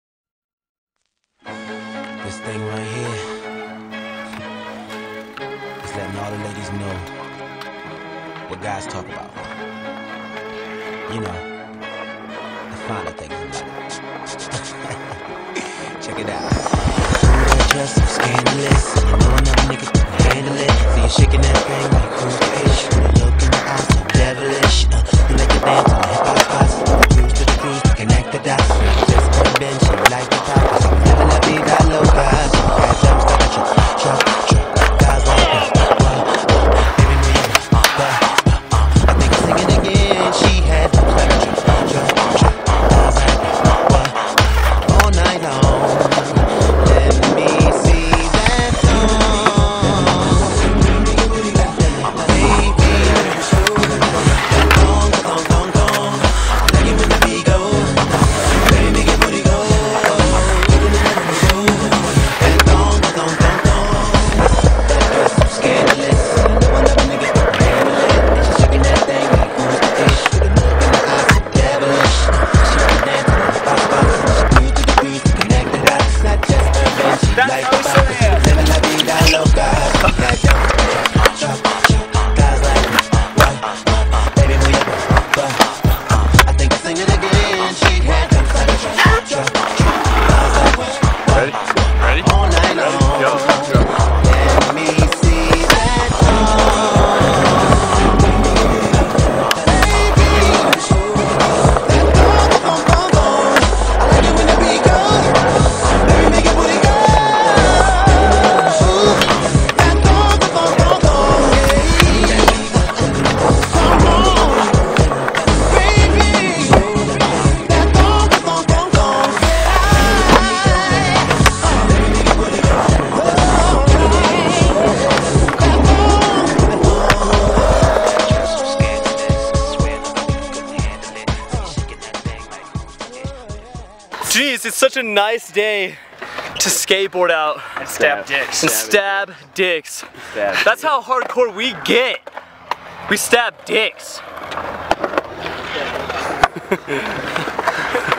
Thornton Skatepark 9-22-12
Bangarang boys headed south to Thornton skatepark for a day of fun, adventure, and dick stabbing skating.